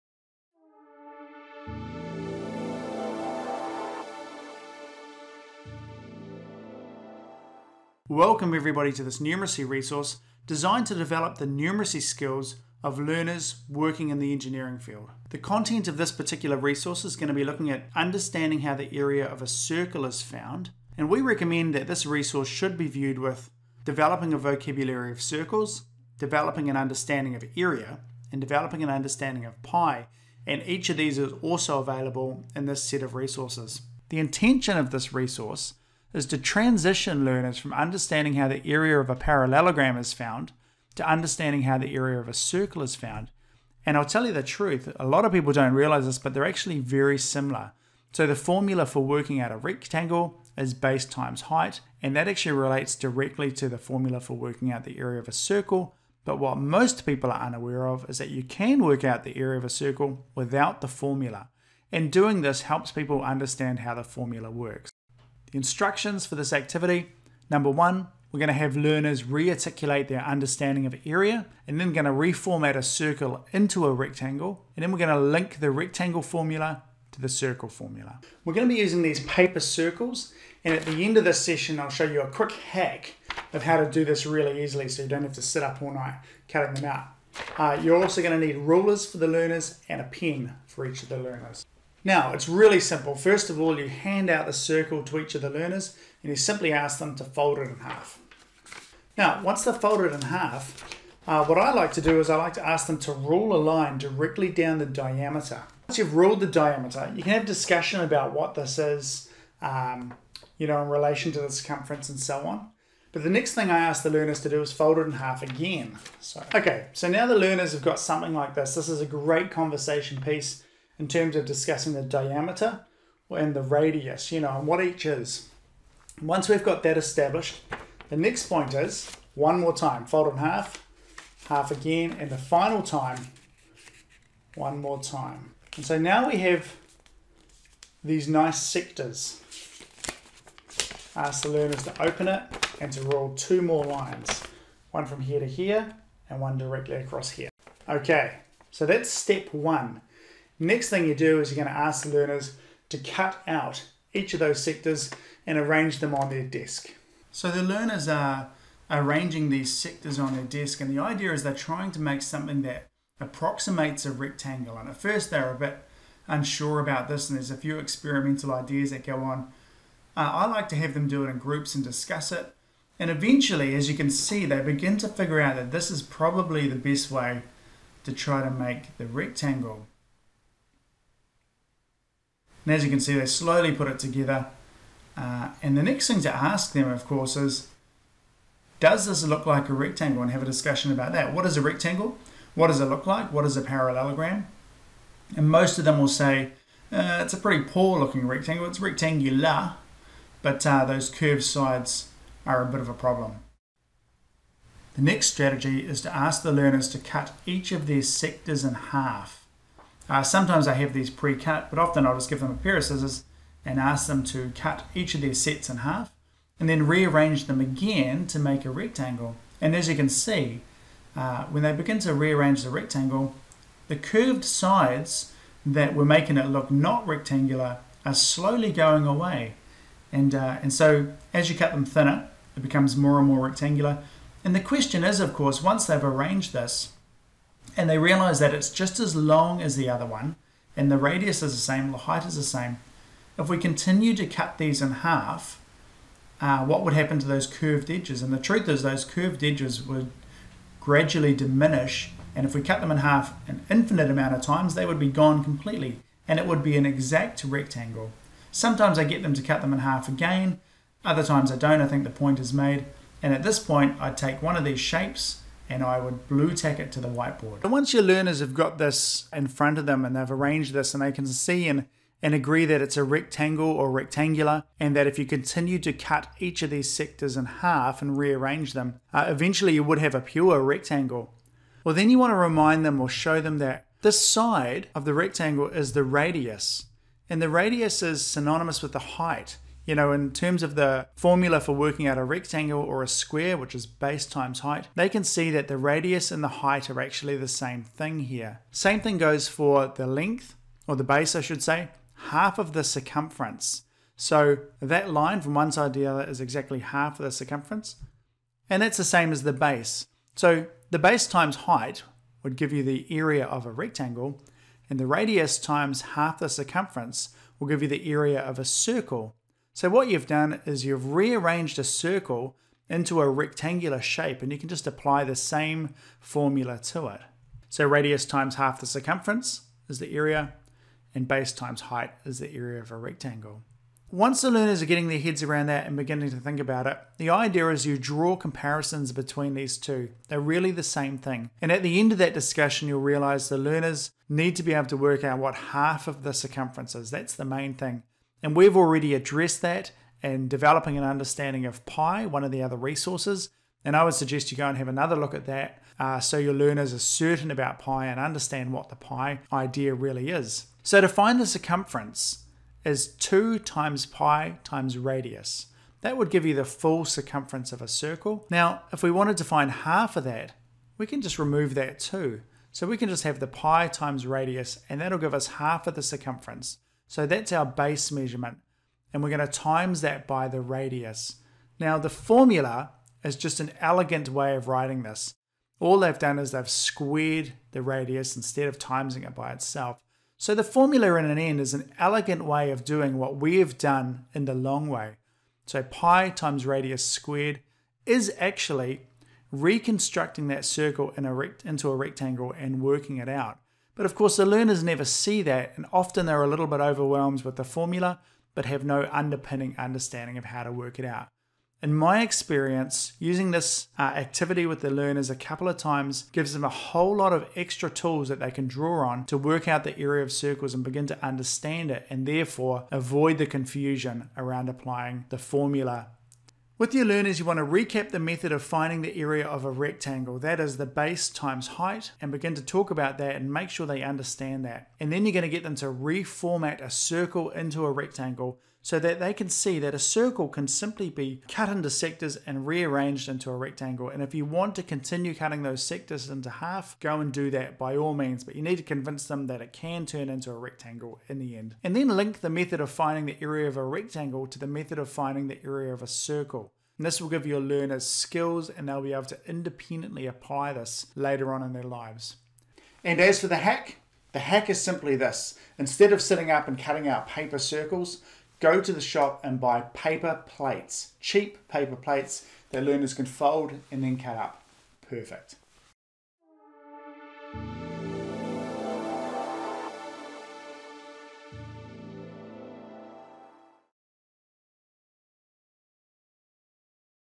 Engineering Developing an understanding of the area of circles
This video is part of a suite of Engineering resources that supports some key areas which educators and learners can find challenging.

 - The metric measurement system
 - Area and perimeter
 - The Pythagoras Theorem

to find the teaching and learning sequences and supporting resources which can be used both as professional learning for educators and trainers and as practical strategies to use with learners.